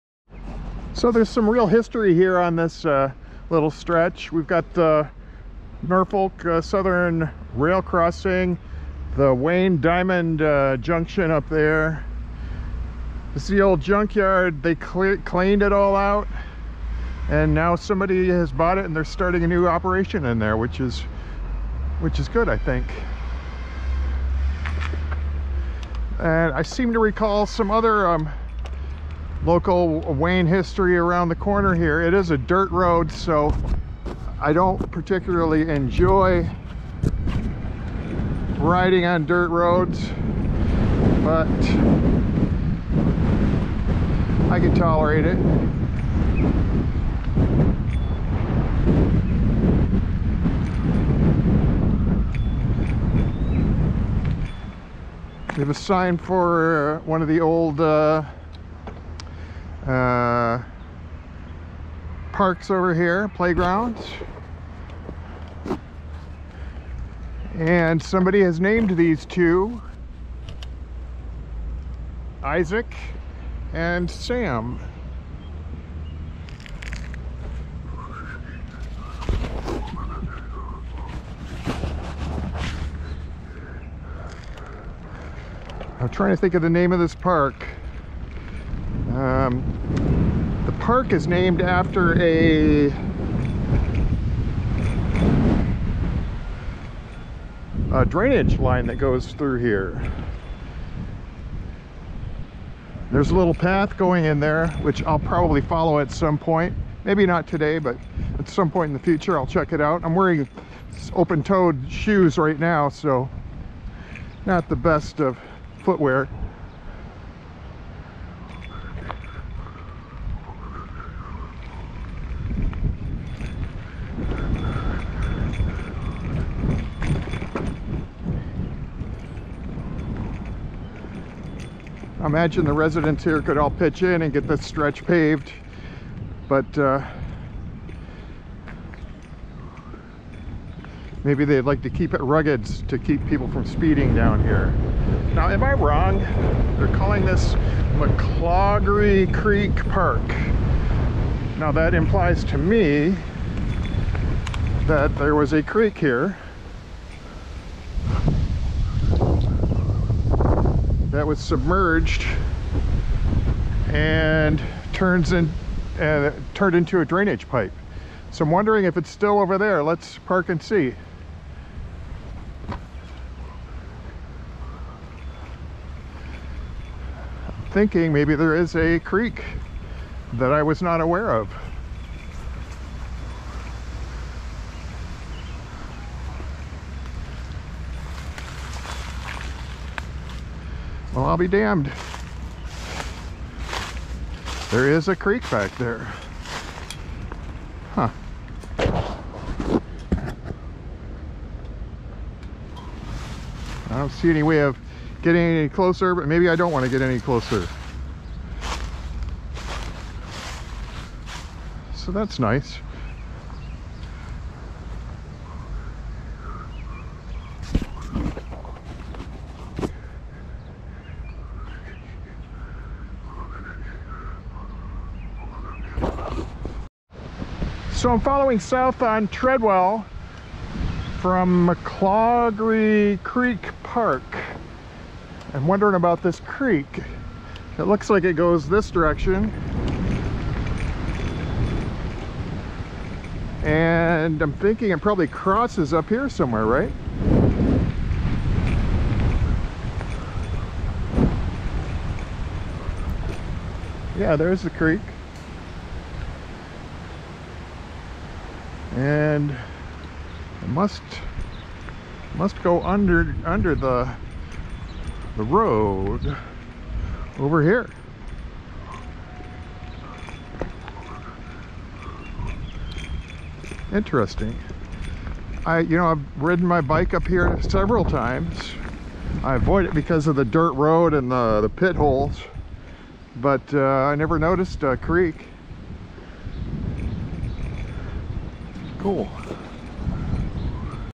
Isaac & Sam, McClaughrey Creek, Treadwell 2024-05-13
VIDEO LINK: Isaac & Sam, McClaughrey Creek, Treadwell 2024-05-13 - Short video starting on Annapolis in front of the old scrap yard that was cleaned out and recently re-stocked; I pause at the diamond-shaped "Playground" sign with see-saw pictogram people that somebody has named "Isaac" and "Sam." Then, I ride West to Treadwell to examine McClaughrey Park, named for the creek that runs parallel to it and goes under Treadwell a little further South. Much of the creek (North of Annapolis up towards the tracks) was submerged at some point and turned into a drainage channel by the same name, but it pops up to become visible here. In dryer weather I man explore North along it's path through the wooded area leading up to the tracks, as there seems to be a path.